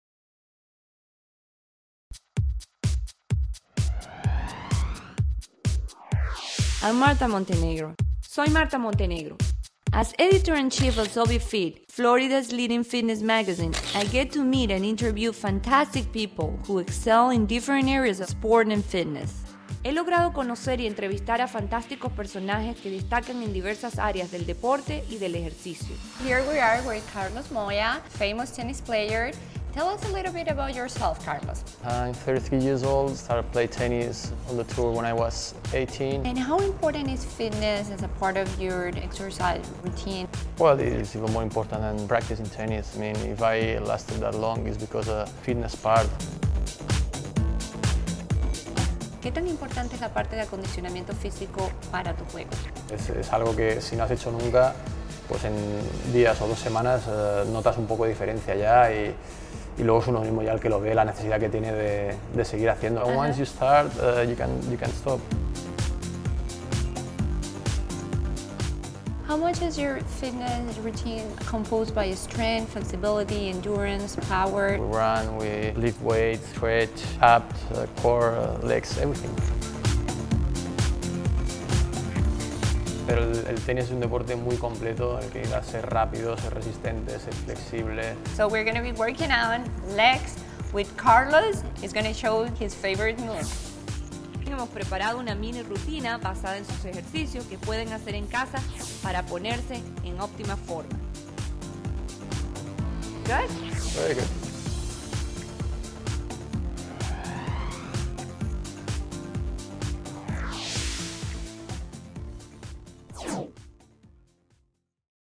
Carlos Moya interview/workout in the Montenegro Method DVD
A preview of Marta Montenegro's interview and mini-workout with Carlos Moya, one of the greatest-ever players from Spain.